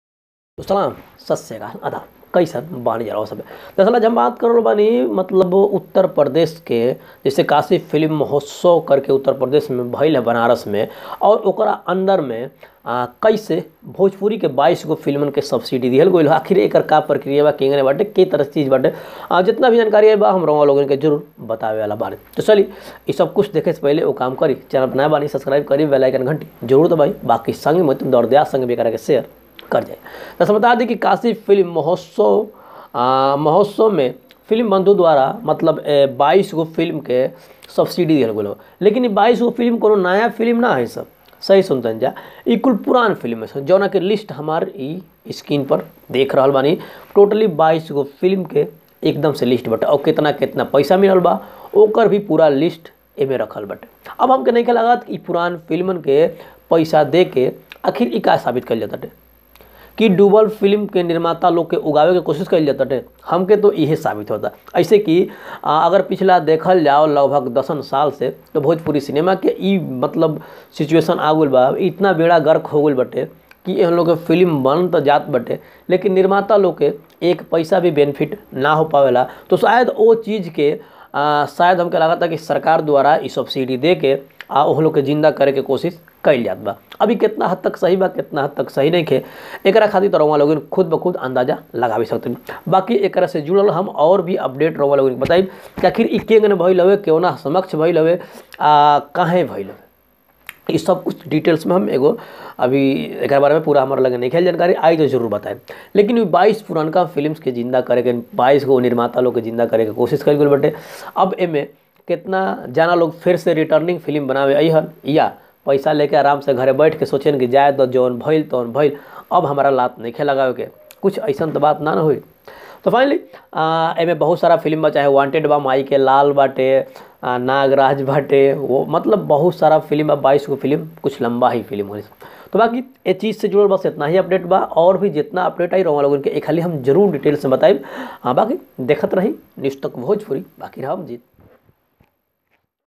पूरानी फिल्मो को क्यो मिल रहा है अनुदान? देखिए बड़ी रिपोर्ट | Pawan, Chintu, Akshra | Jeet Kumar
पूरानी फिल्मो को क्यो मिल रहा है अनुदान? देखिए बड़ी रिपोर्ट | Pawan, Chintu, Akshra | Jeet Kumar | NewsTakBhojpuri

पूरानी फिल्मो को क्यो मिल रहा है अनुदान? देखिए बड़ी रिपोर्ट